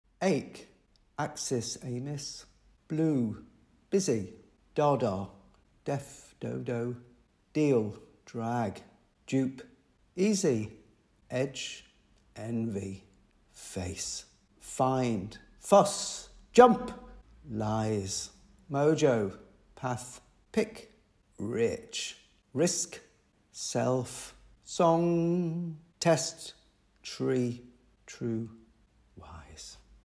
Four letter words June 23
A daily artwork comprising a painting of a four letter word, posted on Instagram. The word, with a brief comment is provoked by something seen, found or heard.